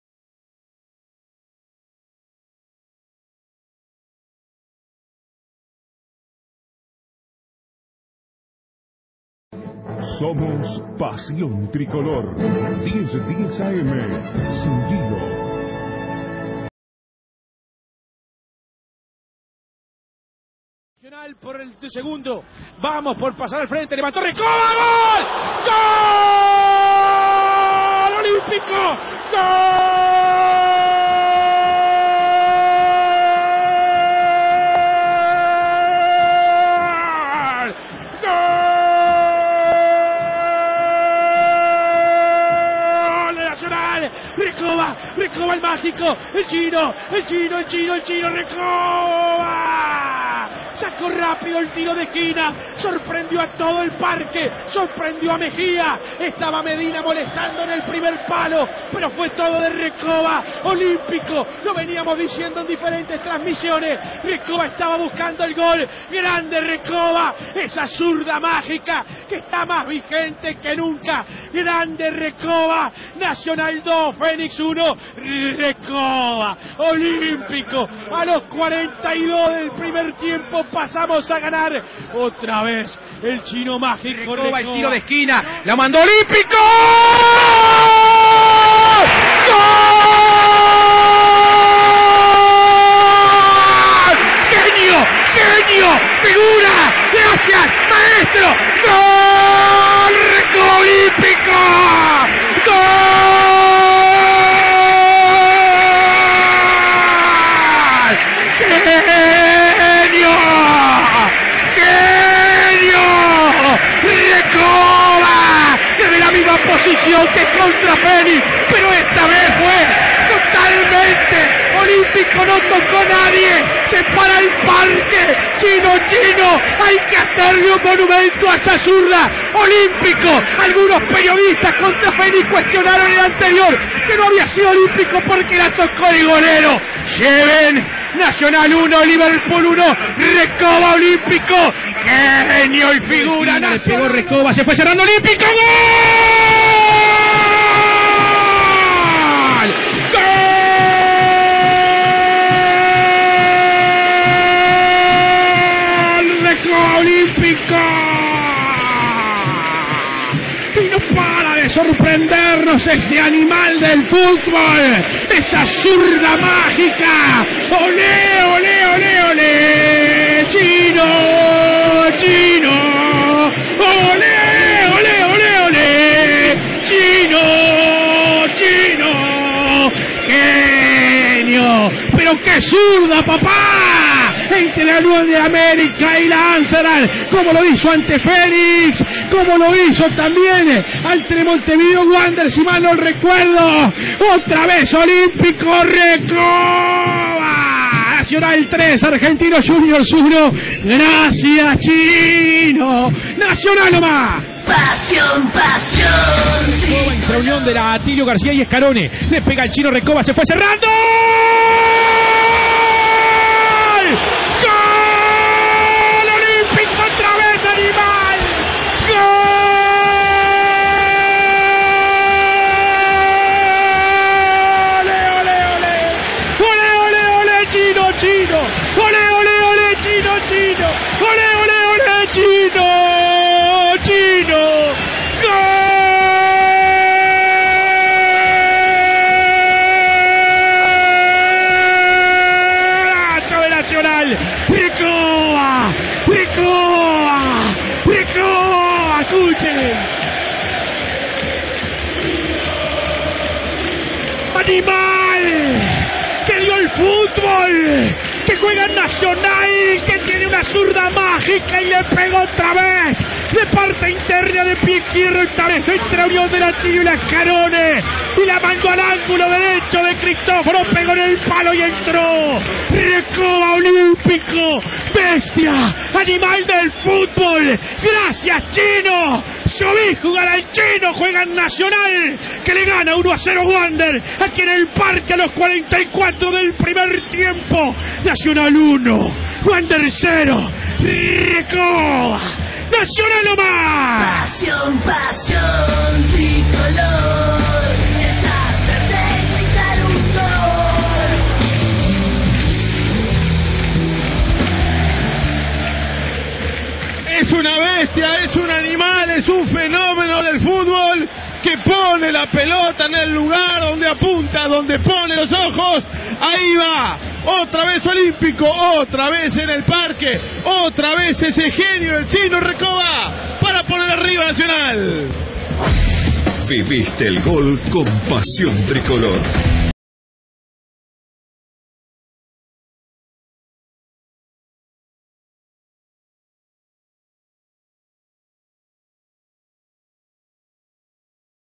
Álvaro Recoba - Los olímpicos
Reviví los cuatro goles olímpicos de el chino Álvaro Recoba en la voz de Javier Moreira con corazón Tricolor